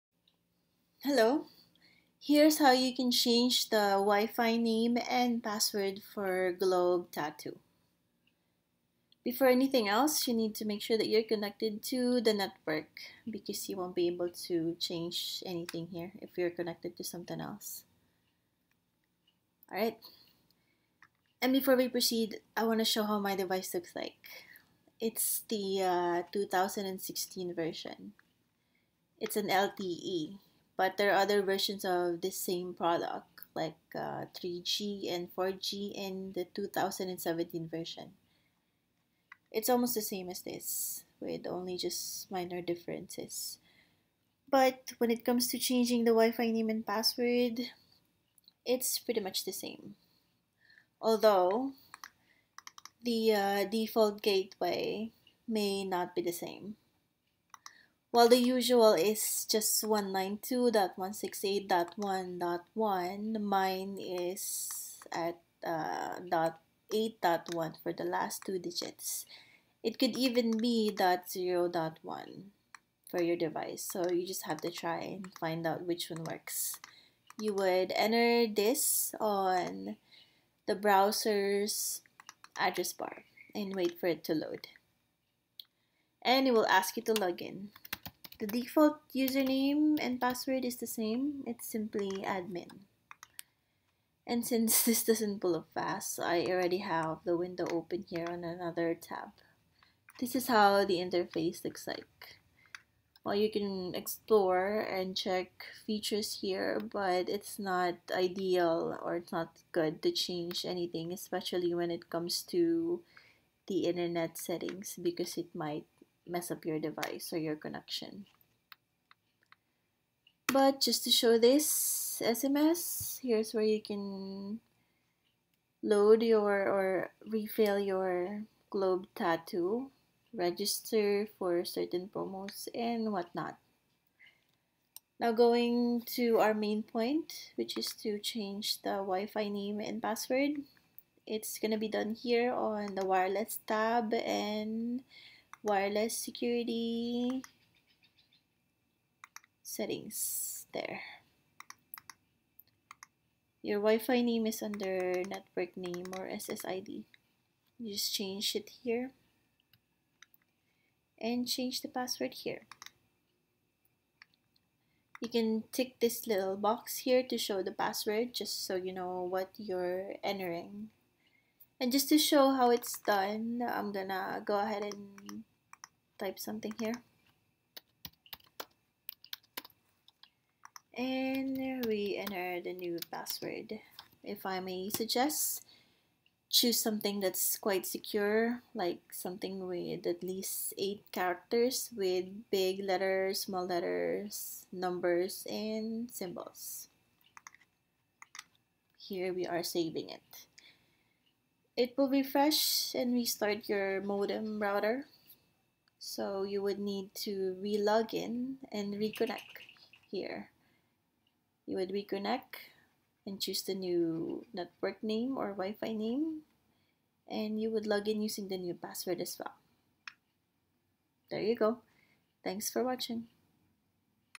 How to Change Globe Tattoo Pocket WiFi Name and Password
Here's a tutorial video on how to change Globe Tattoo pocket WiFi name and password.

For this video, I used the 2016 LTE version.